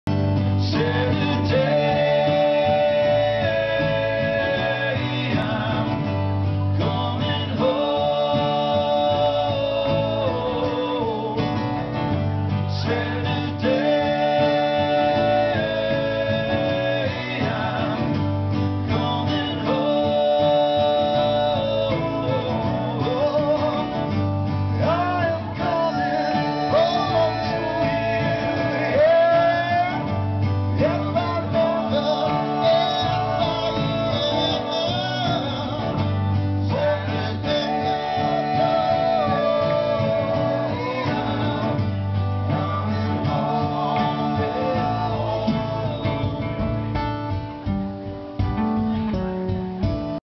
Jay Nash at the 2007 Vail Film Festival
Jay Nash and friends (Cary Brothers, Meiko and Carson Cohen) perform at the Vail Film Festival's "Music Cafe" at the Red Lion.